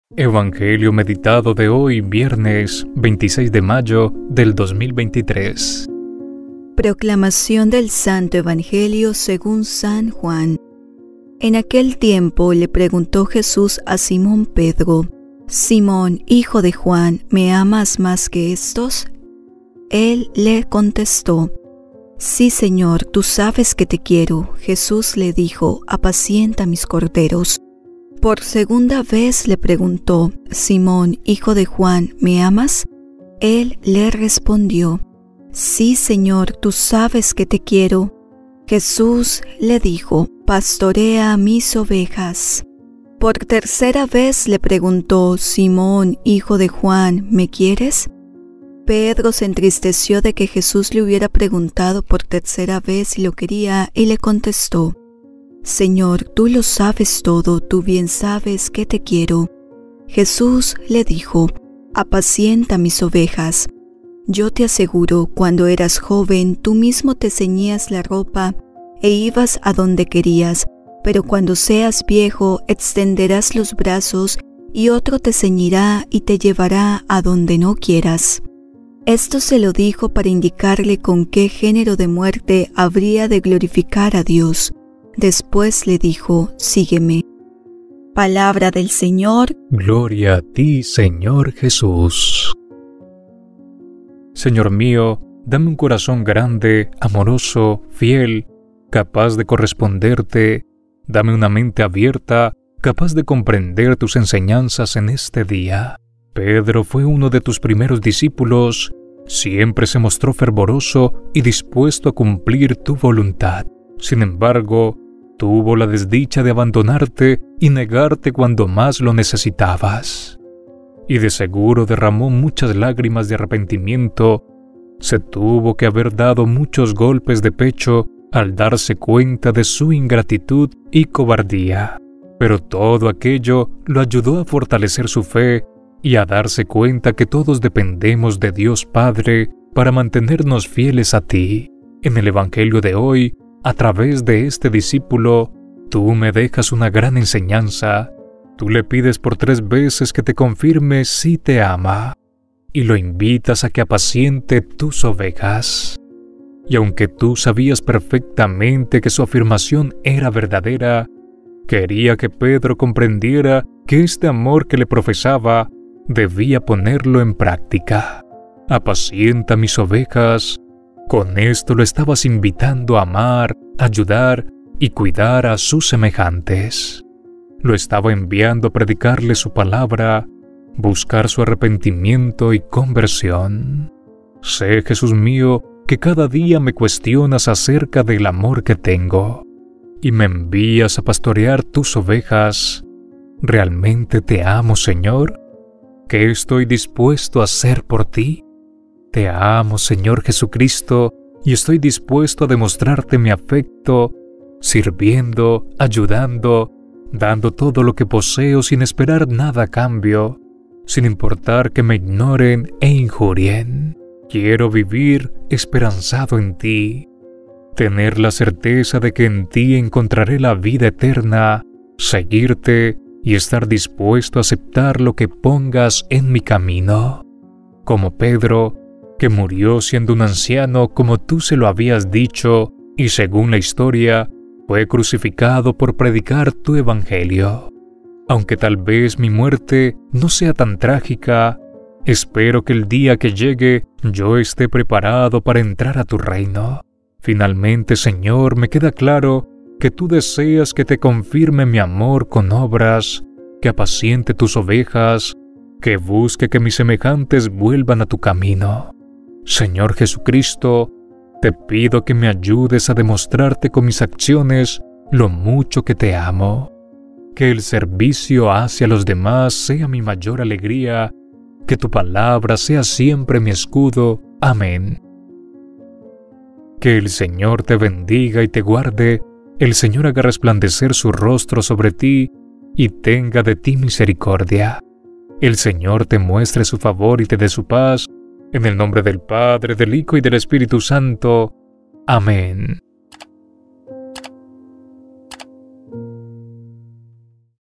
📜 EVANGELIO MEDITADO DE HOY 🙏🏼 VIERNES 26 DE MAYO DE 2023
Hoy viernes queremos compartir el Santo Evangelio de este día  junto a una pequeña meditación, para que orando la Palabra Divina nos esforcemos cada día en ponerla en practica.
Dios los bendiga por orar junto a nosotros y junto a nuestra Madre del Rosario.  VIERNES 26 MAYO 2023.